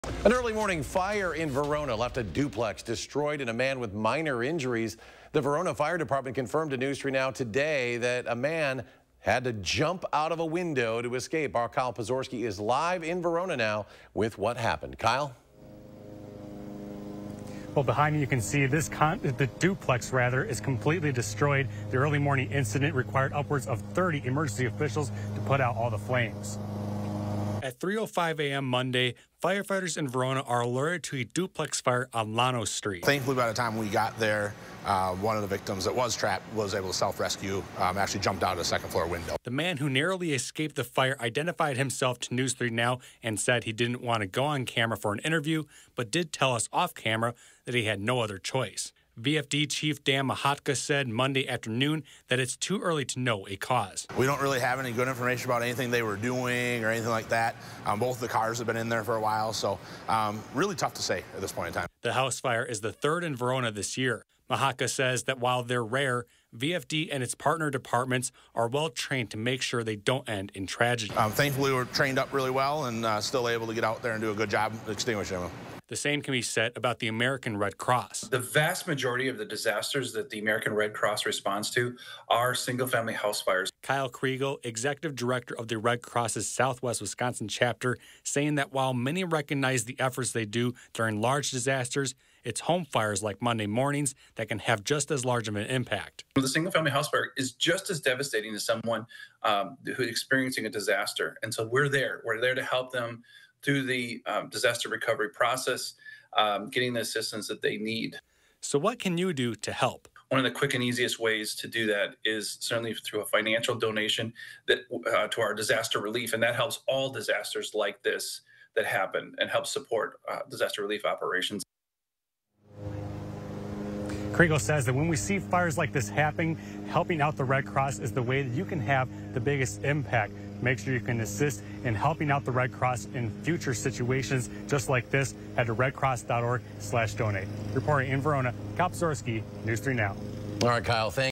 1 injured in Verona duplex fire; building considered total loss
One person was injured Monday morning in a Verona duplex fire; the building is considered a total loss.